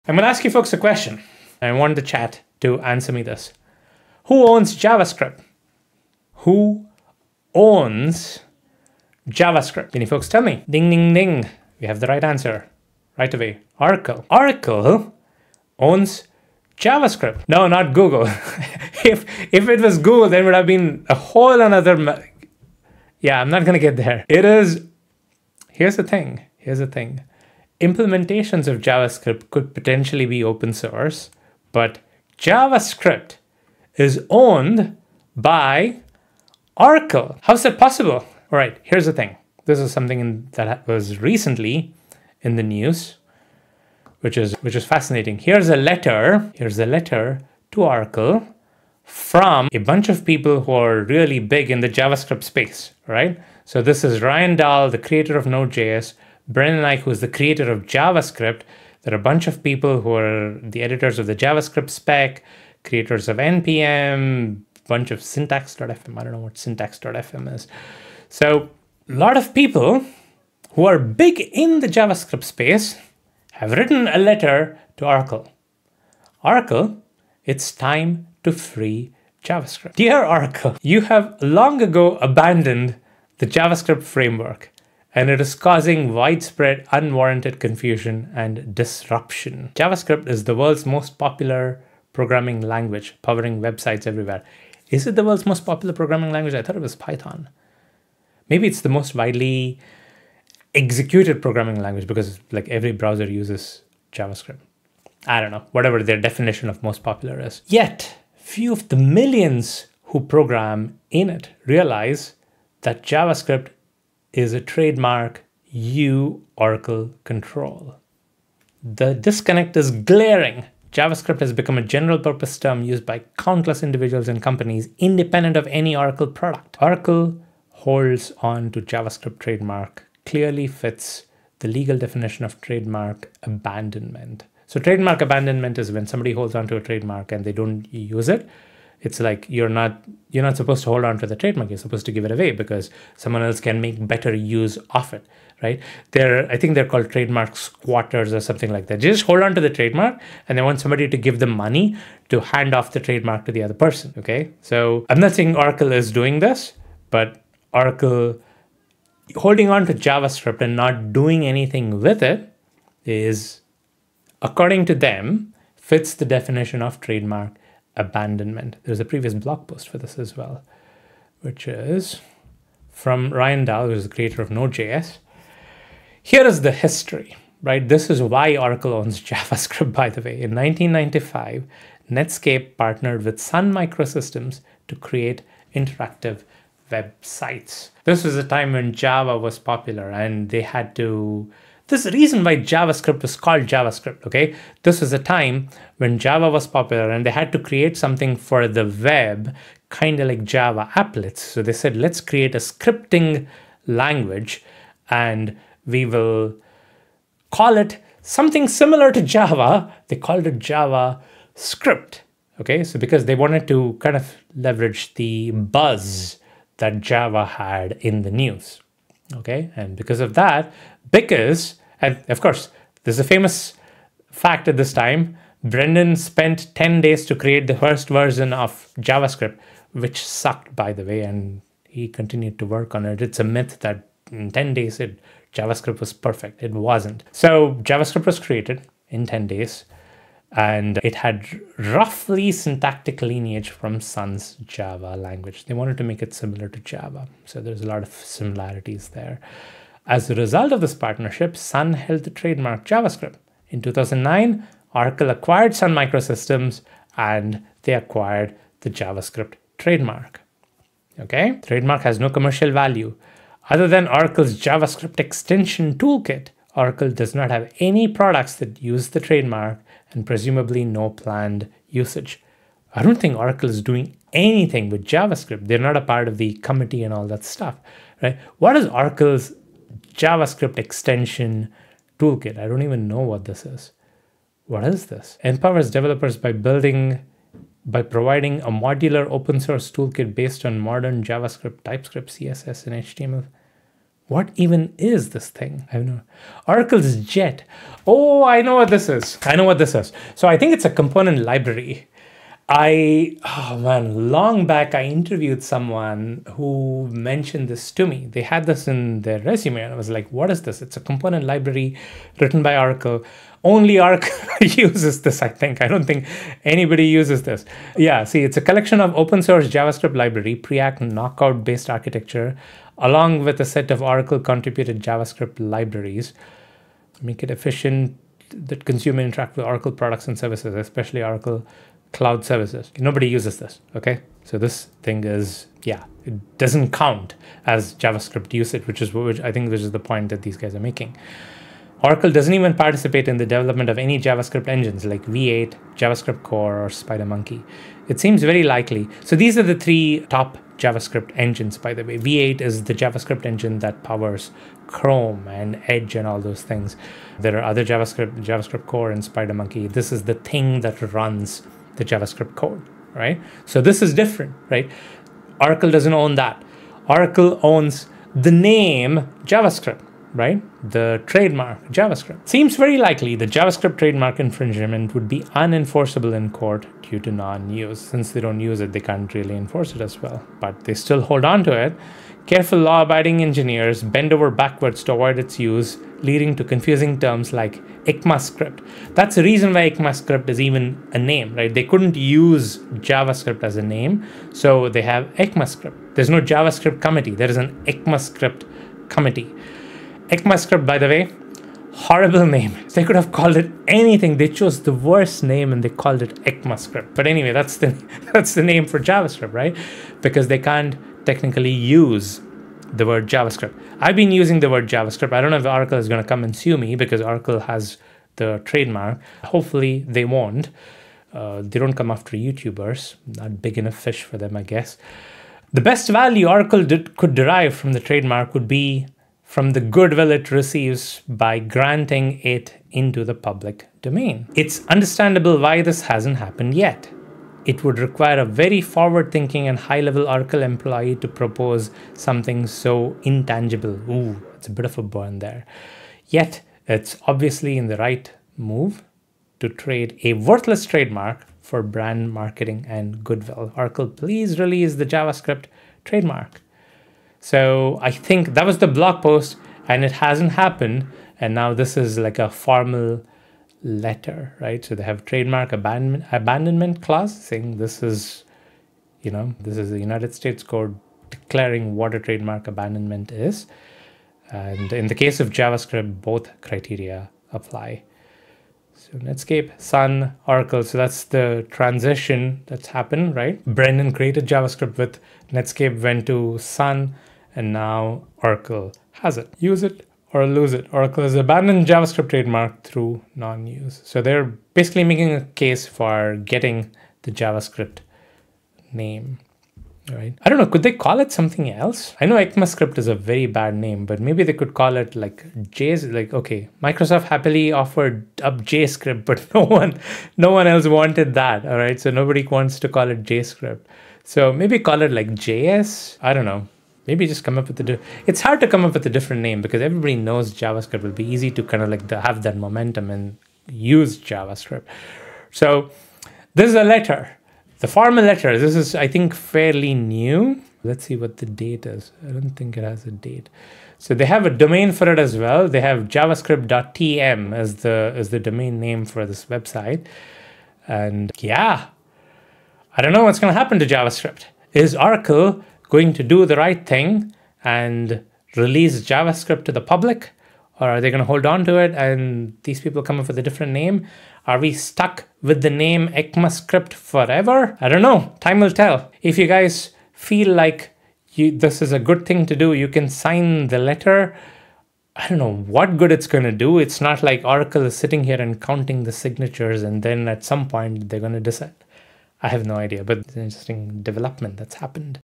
What will happen to JavaScript?
Guess who owns JavaScript? No, I don't mean the open source engines and runtimes. I mean JavaScript itself!

Learn about this wild story involving Netscape, Sun Microsystems, and Oracle. And the latest effort to claim it back!